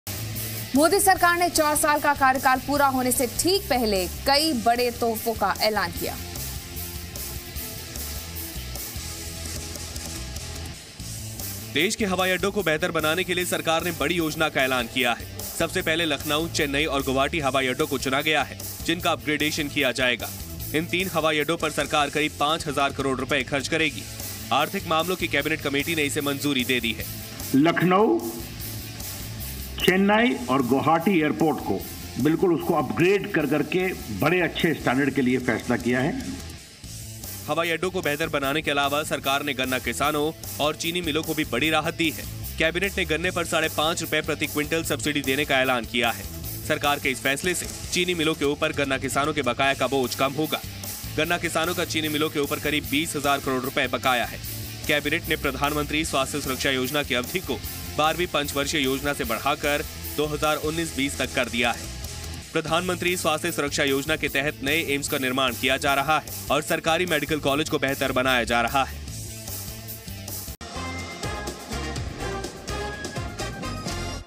Morning Breaking: Lucknow, Chennai, Guwahati airports to get upgraded
Centre has decided to upgrade the airports of Lucknow, Chennai and Guwahati . Watch to know more.